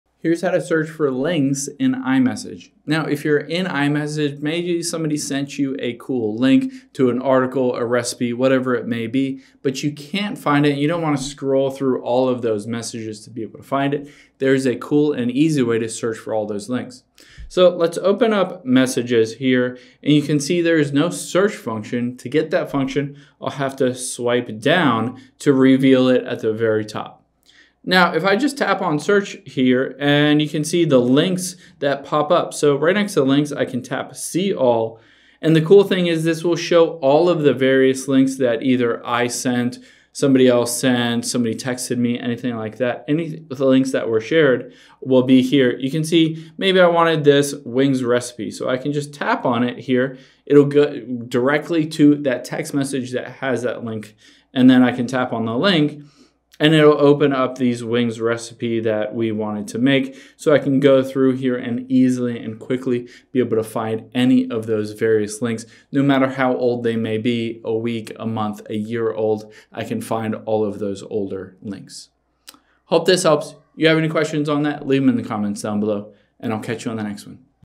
How To Search For Links In iMessage
Here's how you can search for any past links sent or received in your iMessage texts if you want to go to them again.